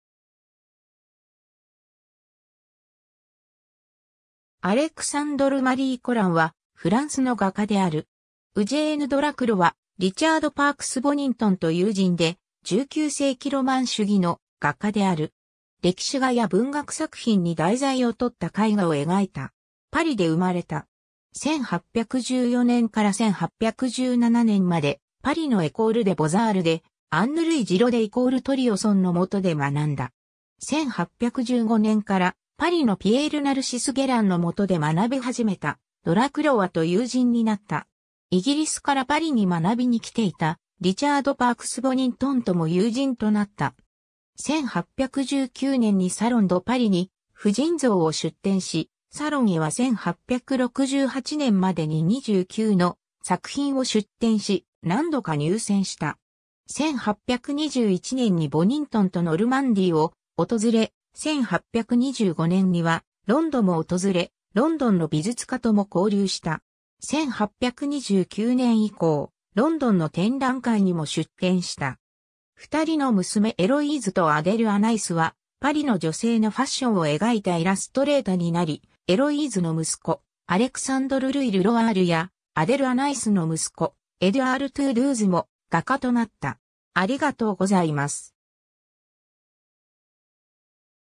アレクサンドル＝マリー・コラン
アレクサンドル＝マリー・コラン（Alexandre-Marie Colin、1798年12月5日 - 1875年11月21日）はフランスの画家である。
ウジェーヌ・ドラクロワ(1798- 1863) 、リチャード・パークス・ボニントン(1802–1828)と友人で、19世紀ロマン主義の画家である。
歴史画や文学作品に題材をとった絵画を描いた。
1814年から1817年まで、パリのエコール・デ・ボザールで、アンヌ＝ルイ・ジロデ＝トリオソンのもとで学んだ。
1815年からパリのピエール＝ナルシス・ゲランのもとで学び始めたドラクロワと友人になった。
イギリスからパリに学びにきていたリチャード・パークス・ボニントンとも友人となった。
 1819年にサロン・ド・パリに婦人像を出展し、サロンへは1868年までに29の作品を出展し、何度か入選した。
 1821年にボニントンとノルマンディーを訪れ、1825年にはロンドも訪れ、ロンドンの美術家とも交流した。
1829年以降、ロンドンの展覧会にも出展した。
 2人の娘エロイーズ（Héloïse Colin）とアデル＝アナイス（Adele-Anaïs Colin Toudouze）はパリの女性のファッションを描いたイラストレータになり、エロイーズの息子、アレクサンドル・ルイ・ルロワールやアデル＝アナイスの息子、エドゥアール・トゥードゥーズも画家となった。